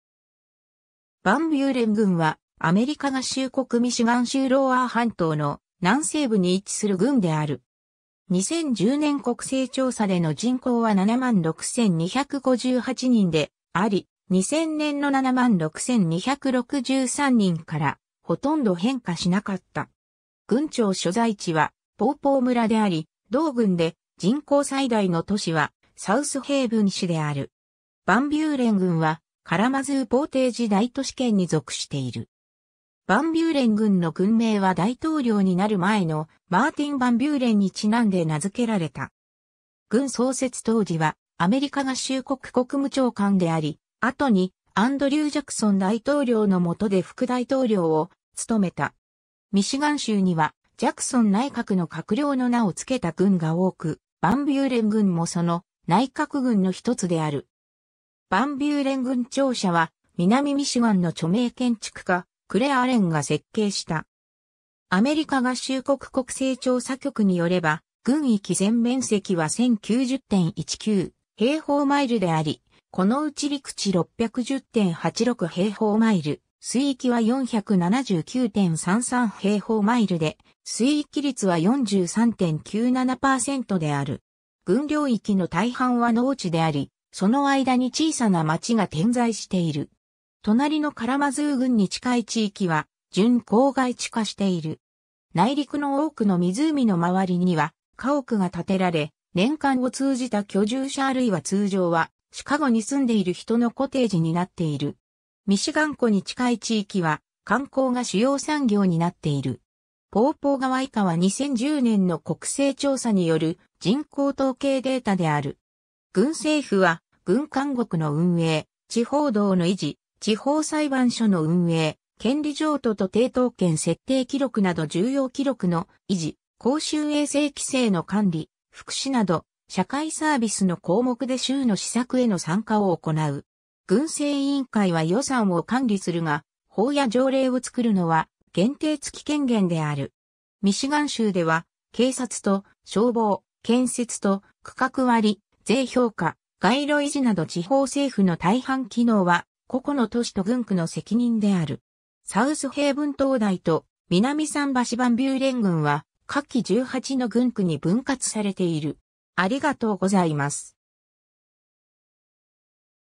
ヴァンビューレン郡 (ミシガン州)
ヴァンビューレン郡（英: Van Buren County）は、アメリカ合衆国ミシガン州ロウアー半島の南西部に位置する郡である。
2010年国勢調査での人口は76,258人であり、2000年の76,263人からほとんど変化しなかった。
郡庁所在地はポーポー村（人口3,534人）であり、同郡で人口最大の都市はサウスヘイブン市（人口4,403人）である。
 ヴァンビューレン郡はカラマズー・ポーテージ大都市圏に属している。
 ヴァンビューレン郡の郡名は大統領になる前のマーティン・ヴァン・ビューレンに因んで名付けられた。
郡創設当時はアメリカ合衆国国務長官であり、後にアンドリュー・ジャクソン大統領の下で副大統領を務めた。
ミシガン州にはジャクソン内閣の閣僚の名をつけた郡が多く、ヴァンビューレン郡もその「内閣郡」の1つである。
 ヴァンビューレン郡庁舎は、南ミシガンの著名建築家クレア・アレンが設計した。
 アメリカ合衆国国勢調査局に拠れば、郡域全面積は1,090.19平方マイル (2,823.6 km2)であり、このうち陸地610.86平方マイル (1,582.1 km2)、水域は479.33平方マイル (1,241.5 km2)で水域率は43.97%である。
 郡領域の大半は農地であり、その間に小さな町が点在している。
隣のカラマズー郡に近い地域は準郊外地化している。
内陸の多くの湖の周りには家屋が建てられ、年間を通じた居住者あるいは通常はシカゴに住んでいる人のコテージになっている。
ミシガン湖に近い地域は観光が主要産業になっている。
 ポーポー川 以下は2010年の国勢調査による人口統計データである。
 郡政府は郡監獄の運営、地方道の維持、地方裁判所の運営、権利譲渡と抵当権設定記録など重要記録の維持、公衆衛生規制の管理、福祉など社会サービスの項目で州の施策への参加を行う。
郡政委員会は予算を管理するが、法や条例を作るのは限定付き権限である。
ミシガン州では、警察と消防、建設と区画割、税評価、街路維持など地方政府の大半機能は個々の都市と郡区の責任である。
 サウスヘイブン灯台と南桟橋 ヴァンビューレン郡は下記18の郡区に分割されている。